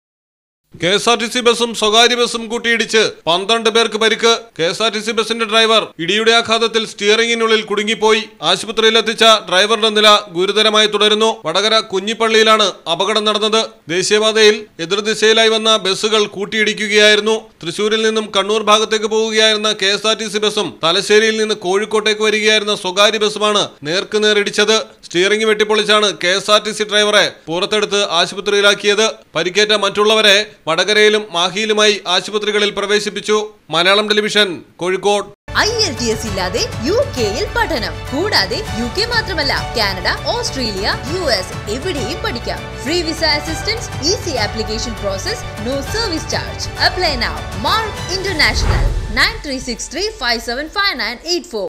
KSRTC, പ്രൈവറ്റ് ബസ്സുകൾ നേർക്കു നേർ ഇടിച്ച് തകർന്നു, നിരവധി യാത്രക്കാർക്ക് പരിക്ക്.
KSRTC,  പ്രൈവറ്റ് ബസ്സുകൾ നേർക്കു നേർ ഇടിച്ച് തകർന്നു, സ്റ്റിയറിങ്ങിനിടയിൽ കുടുങ്ങിയ ഡ്രൈവറുടെ നില ഗുരുതരം, നിരവധി യാത്രക്കാർക്ക് പരിക്ക്.